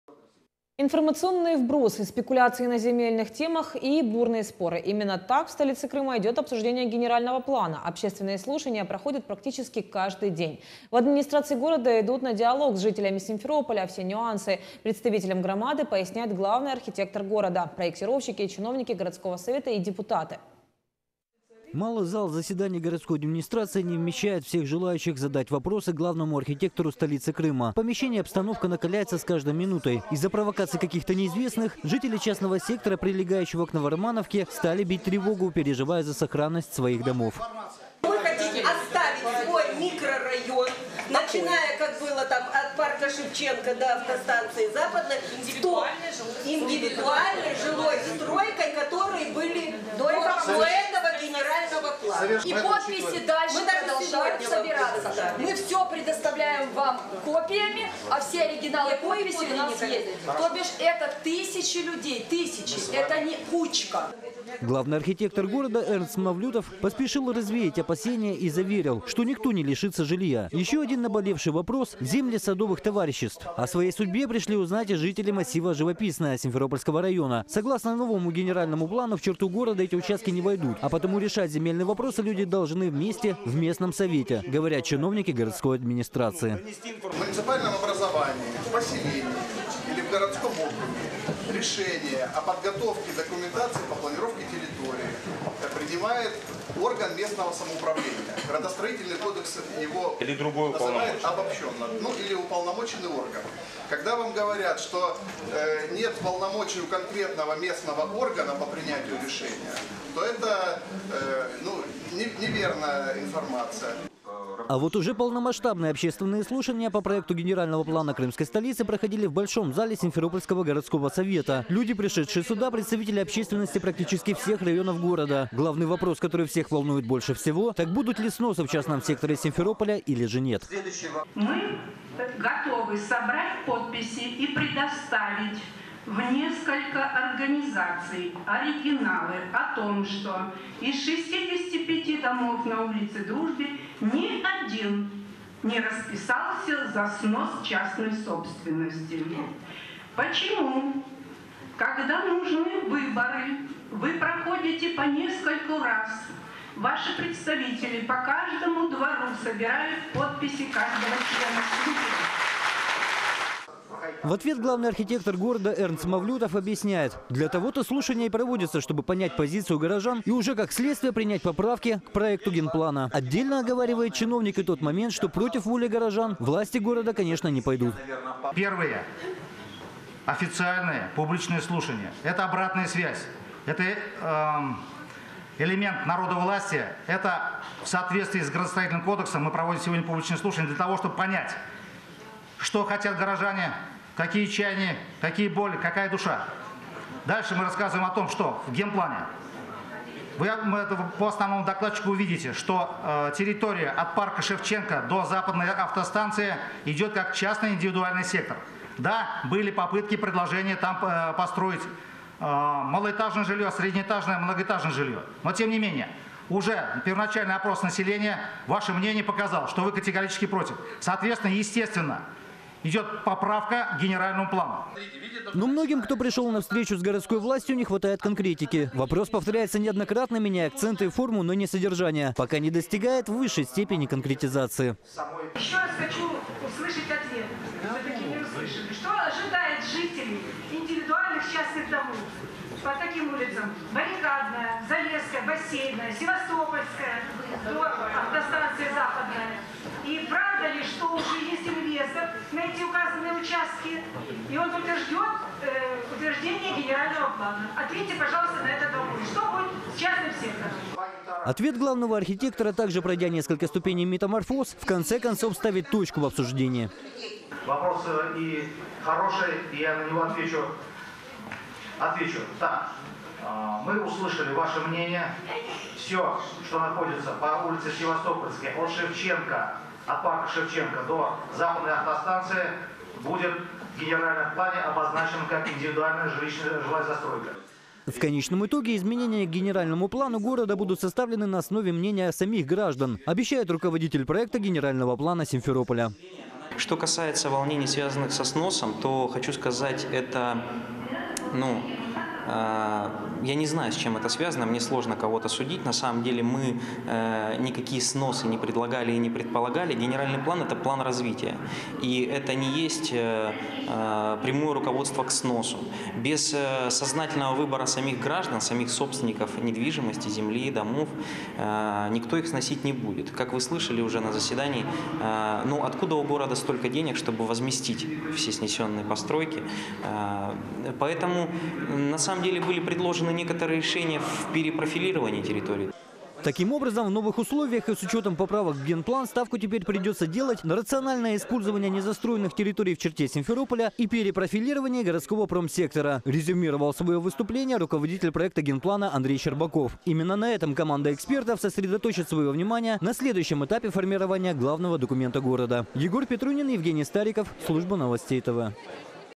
Сюжет – В столице Крыма идет обсуждение генерального плана
Информационные вбросы, спекуляции на земельных темах и бурные споры – именно так в столице Крыма идет обсуждение генерального плана. Общественные слушания проходят практически каждый день. В администрации города идут на диалог с жителями Симферополя, все нюансы представителям громады поясняет главный архитектор города, проектировщики, чиновники городского совета и депутаты.